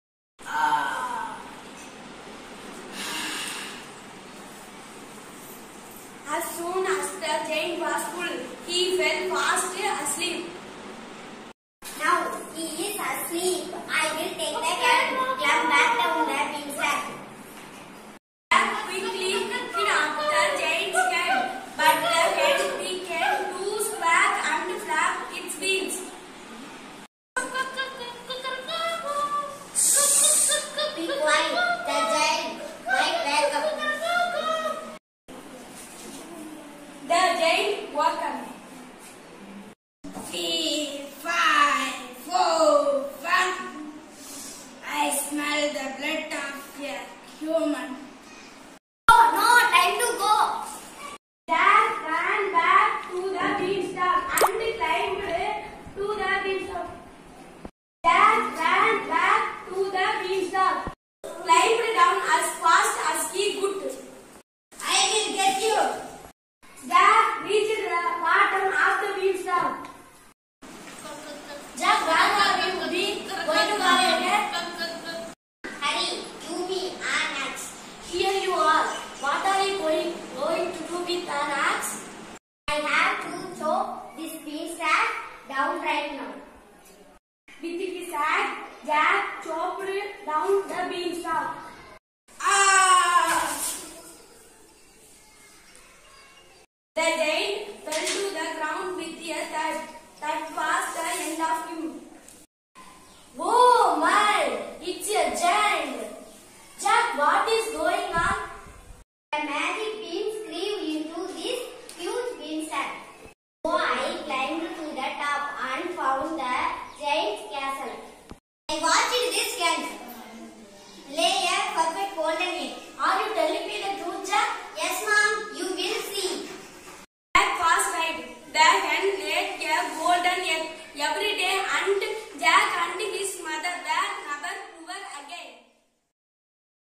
JACK AND THE BEANSTALK SCENE III
Presented by Panchayat Union Middle School Students Melakandamangalam Kottur Block Thiruvarur Dt Tamilnadu India